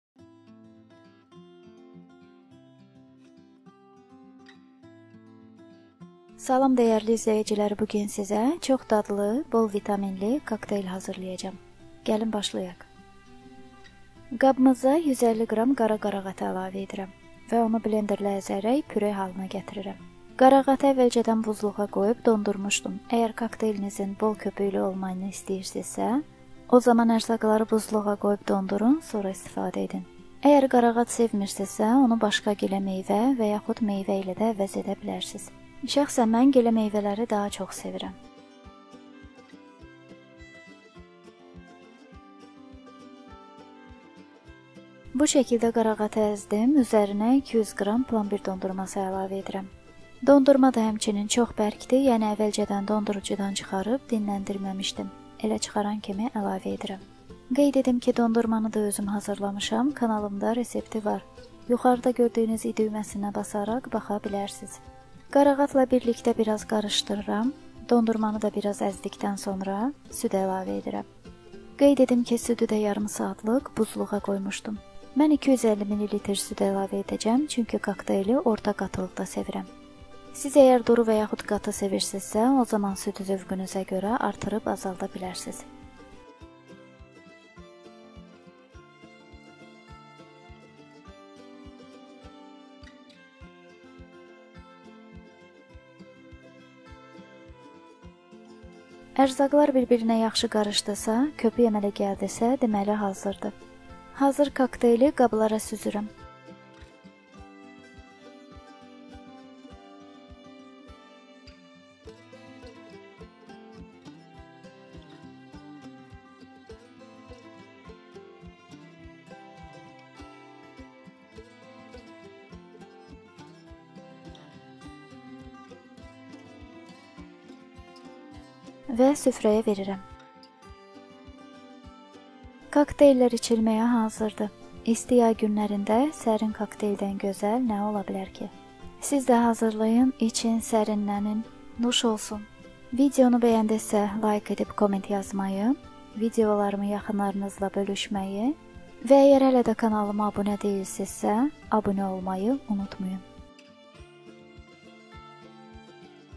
Sudlu kokteyl- молочный коктейль
Sudlu kokteyl
Lazim olan erzaqlar
250 ml-sud(1 st)
200 qr-dondurma
150 qr-qara qaraqat